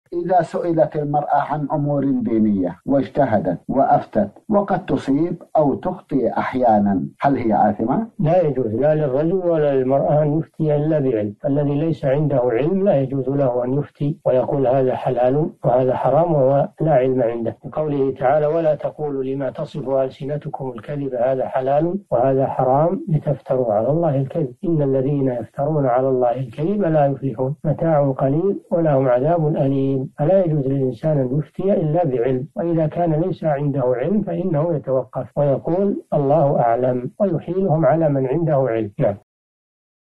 إذا سئلت المرأة عن أمور دينية واجتهدت وأفتت وقد تصيب أو تخطئ أحيانا، هل هي آثمة؟ الشيخ صالح الفوزان
نص السؤال: إذا سُئلت المرأة عن أمور دينية واجتهدت وأفتت وقد تُصيب أو تُخطئ أحيانًا، هل هي آثمة؟
السيرة الذاتية :
للاشتراك في قناة الشيخ صالح الفوزان

رئيس الفريق
لمشروع كبار العلماء